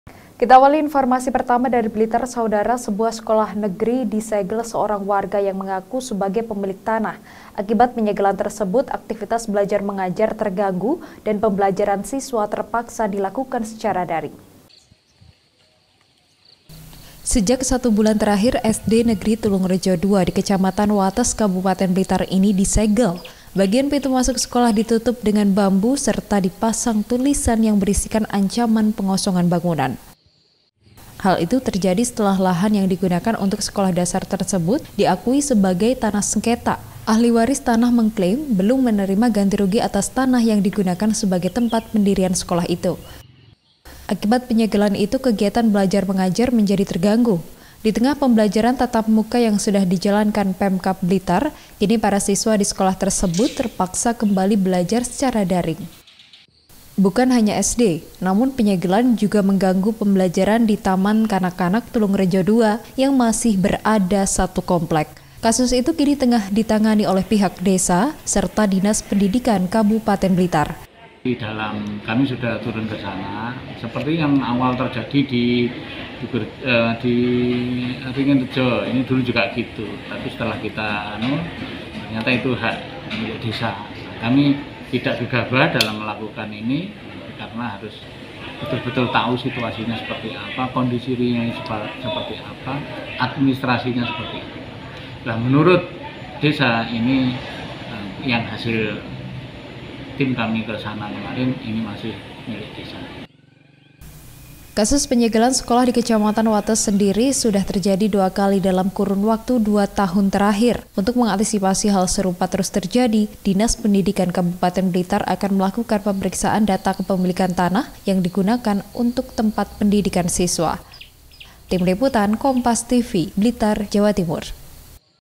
Gedung Sekolah Dasar Disegel Ahli Waris, Kegiatan Belajar Mengajar Terganggu
BLITAR, KOMPAS.TV - Sebuah Sekolah Dasar Negeri di Kabupaten Blitar, disegel oleh seorang warga yang mengaku sebagai pemilik tanah. akibat penyegelan tersebut aktivitas belajar mengajar terganggu dan pembelajaran siswa terpaksa dilakukan secara daring.

Sejak 1 bulan terakhir SD Negeri Tulungrejo 2 di Kecamatan Wates Kabupaten Blitar ini disegel. Bagian pintu masuk sekolah ditutup dengan bambu, serta dipasang tulisan yang berisikan ancaman pengosongan bangunan.

Hal itu terjadi setelah lahan yang gunakan untuk sekolah dasar tersebut diaku sebagai tanah sengketa. Ahli waris tanah mengklaim, belum menerima ganti rugi atas tanah yang digunakan sebagai tempat pendirian sekolah itu.

Akibat penyegelan itu, kegiatan belajar mengajar menjadi terganggu. Di tengah pembelajaran tatap muka yang sudah dijalankan Pemkab Blitar, kini para siswa di sekolah tersebut terpaksa kembali belajar secara daring.

Bukan hanya SD, namun penyegelan juga mengganggu pembelajaran di Taman Kanak-Kanak Tulungrejo 2 yang masih berada satu komplek. Kasus itu kini tengah ditangani oleh pihak desa serta Dinas Pendidikan Kabupaten Blitar.

Kasus penyegelan sekolah di Kecamatan Wates sendiri sudah terjadi 2 kali dalam kurun waktu 2 tahun terakhir. Untuk mengantisipasi hal serupa terus terjadi, Dinas Pendidikan Kabupaten Blitar akan melakukan pemeriksaan data kepemilikan tanah yang digunakan untuk tempat pendidikan siswa.